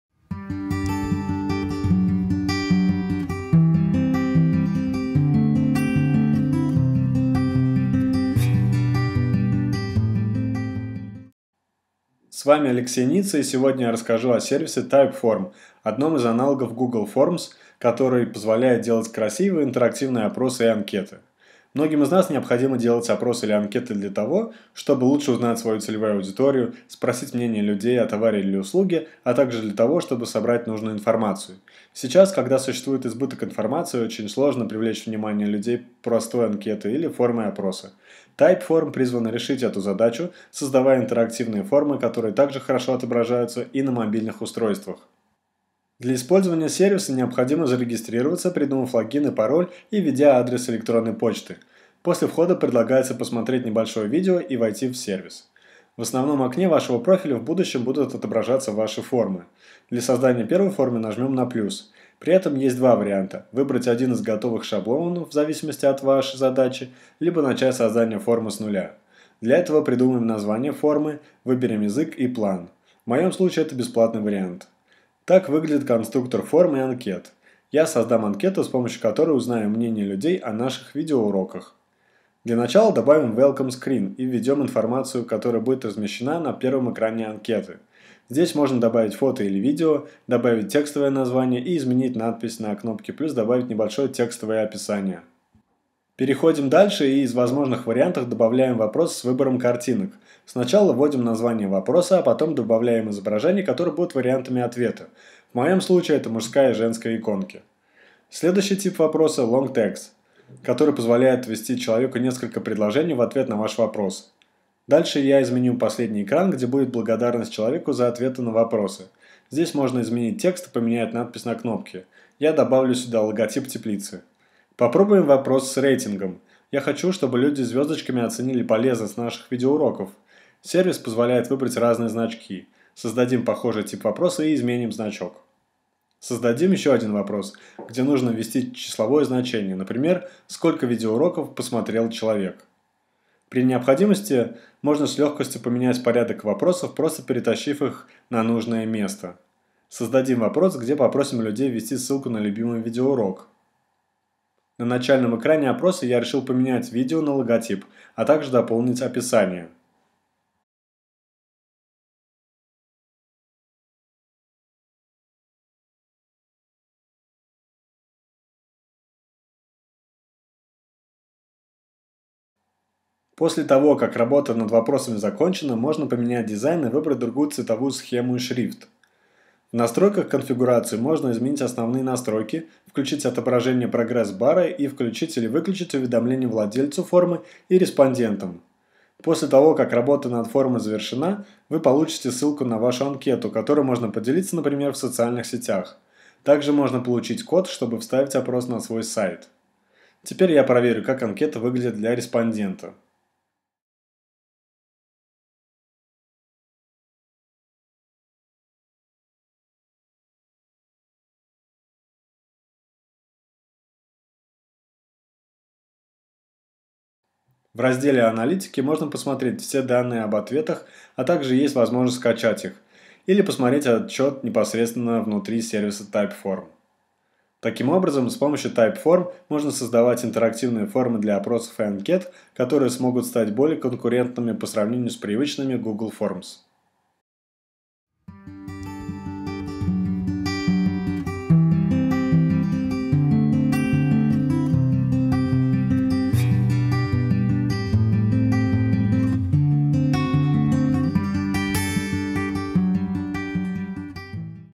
Как создать красивые опросы и анкеты с помощью Typeform
В этом видеоуроке вы узнаете об одном из аналогов Google Форм, который позволяет делать красивые и интерактивные опросы и анкеты.

Многим из нас необходимо делать опросы или анкеты для того, чтобы лучше узнать свою целевую аудиторию, спросить мнение людей о товаре или услуге, а также для того, чтобы собрать нужную информацию.

Сейчас, когда существует избыток информации, очень сложно привлечь внимание людей простой анкетой или формой опроса. Typeform призван решить эту задачу, создавая интерактивные формы, которые также хорошо отображаются и на мобильных устройствах.